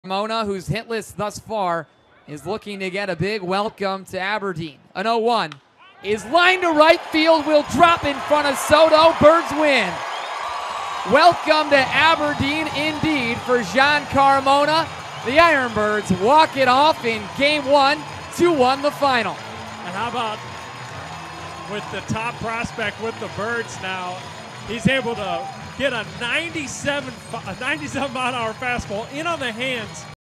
Aberdeen's Carmona knocks in game-winning run
8/5/18: Aberdeen's Jean Carmona knocked in the game-winning run on an RBI single in the ninth as the IronBirds beat Staten Island 2-1